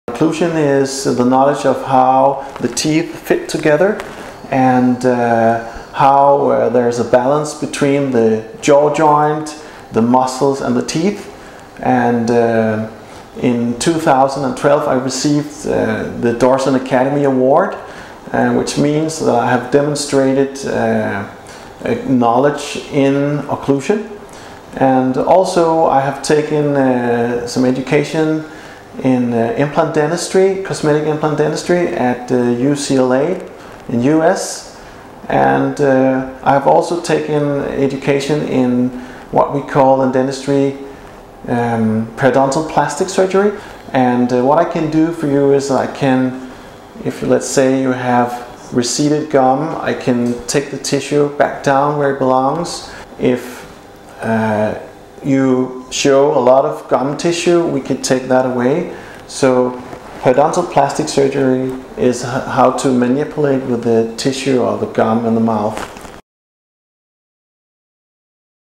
Dr.Onuoha discusses Occlusion & Periodontal Surgery
Our dentist, Dr. Marc Onuoha discusses his knowledge of occlusion and periodontal surgery and how he can help you.

Seapoint Clinic is based on Seapoint Avenue in Blackrock, Co Dublin. We are a full team of specialists and general dentists under one roof here.